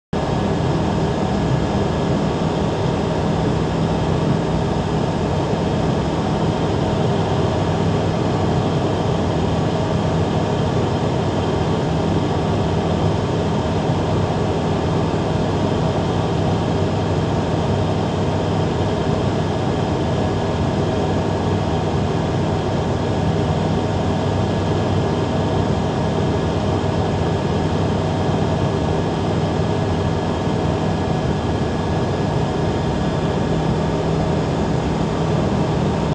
Steep turn practice...
Just out having fun today practicing steep turns...